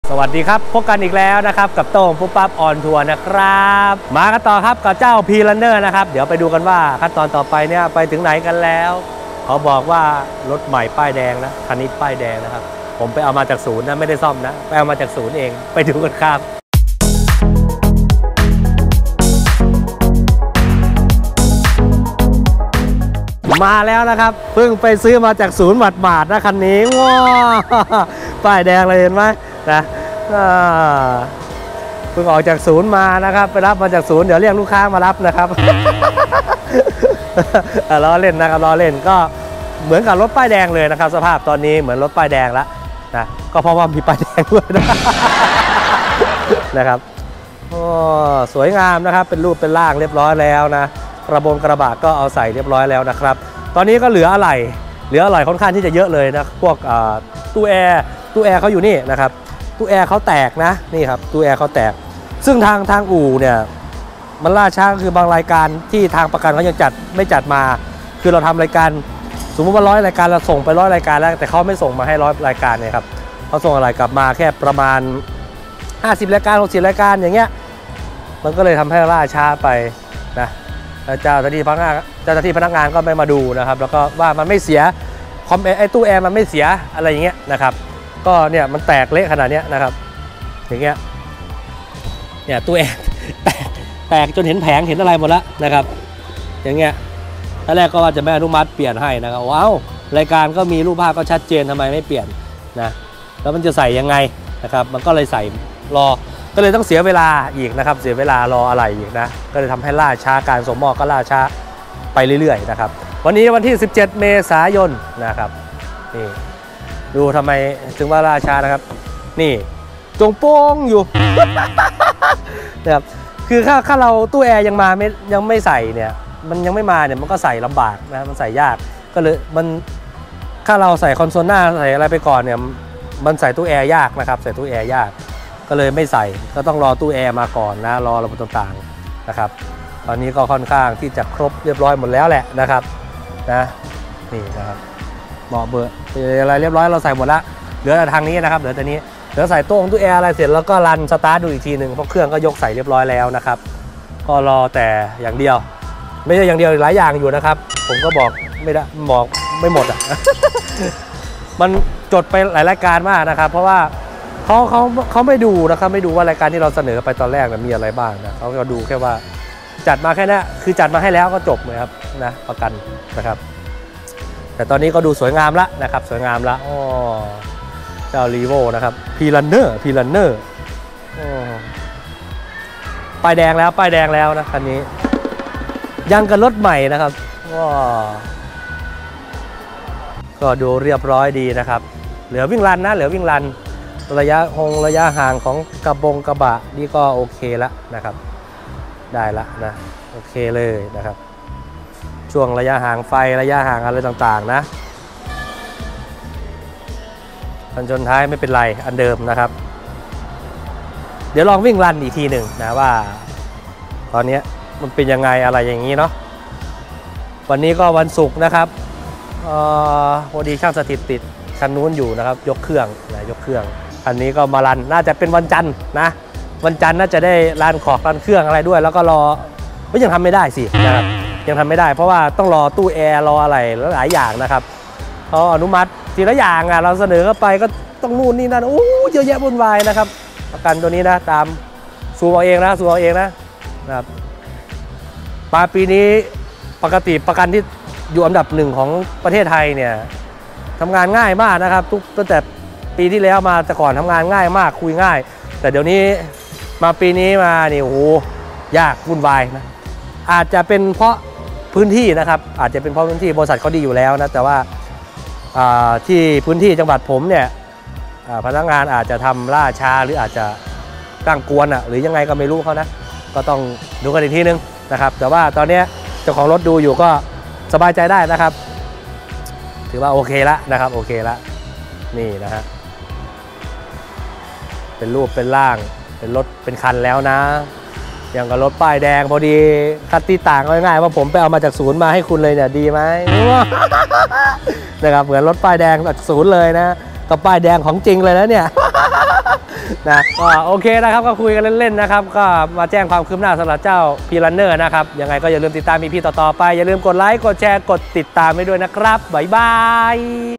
REVO Prerunner ชนแค่นี้ ซ่อมได้ สบายมาก EP11
เป็นรายการเกี่ยวกับ ซ่อมรถ ท่องเทียว ออฟโรด จุดดางเต็นท์ และความรู้เกี่ยวกับยานยนต์